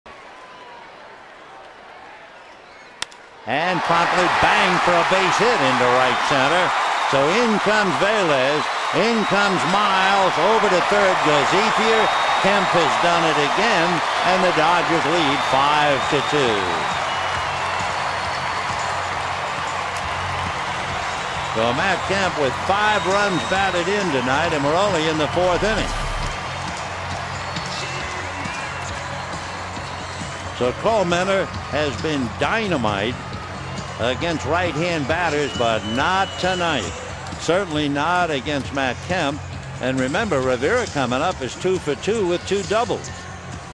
2011/07/29 Kemp's two-run single
ARI@LAD: Kemp drives in a pair to pad lead in fourth